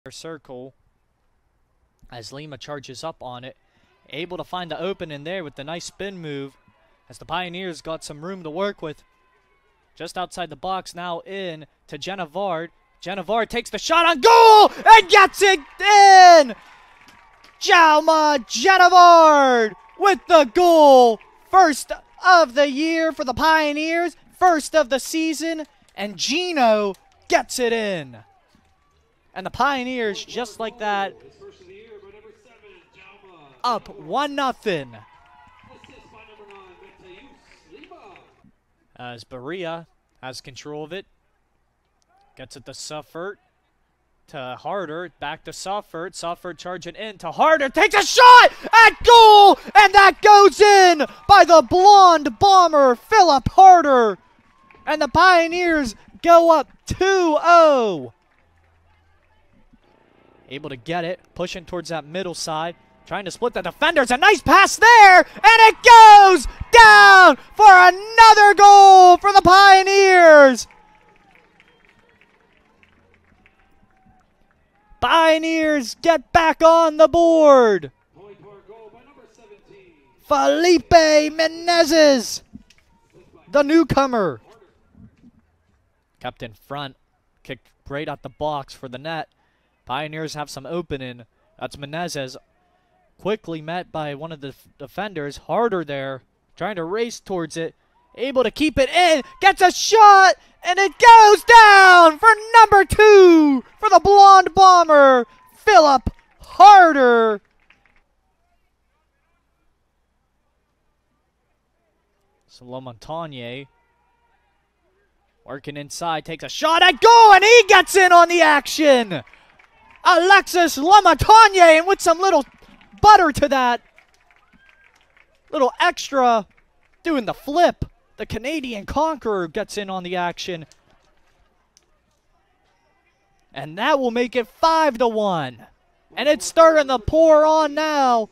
Point Park Men's Soccer Highlights vs. Goshen (Ind.)
Let's take a look at the goals for Point Park University Men's Soccer in a 5-1 win over Goshen (Ind.). Highlights from the Point Park Sports Network. Mike Hodgetts on the call.